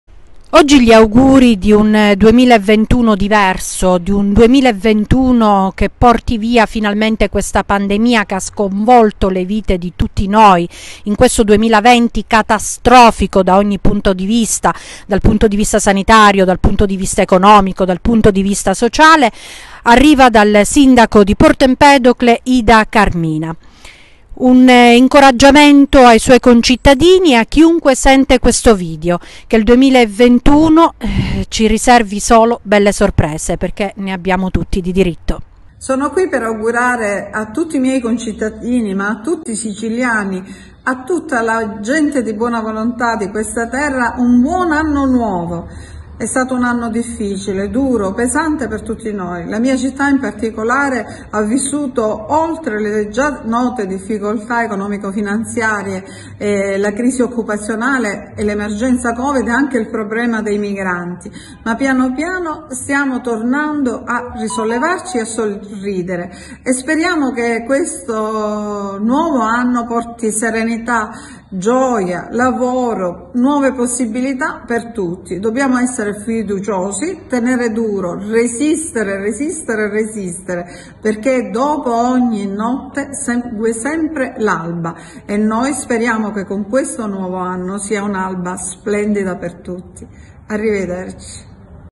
Porto Empedocle, sindaco Carmina: “2021 l’augurio è che sia una nuova alba per tutti”
“Dopo la notte arriva sempre l’alba” è questo l’augurio per il 2021 che il sindaco di Porto Empedocle Ida Carmina rivolge a tutti i siciliani.